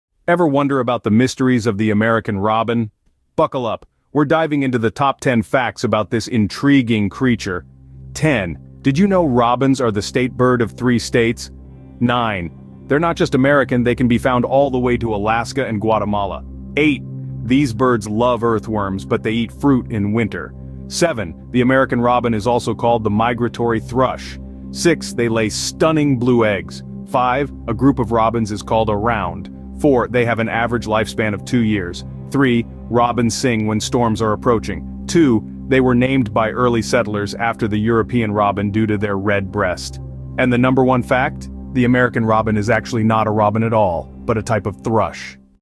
Most Amazing Top 10 Facts About The American Robin
Today we bring you the most amazing top 10 facts about the American robin! These fascinating birds are a common sight across North America, known for their distinctive red breast and cheerful songs. Join us as we uncover some incredible facts about these beloved creatures.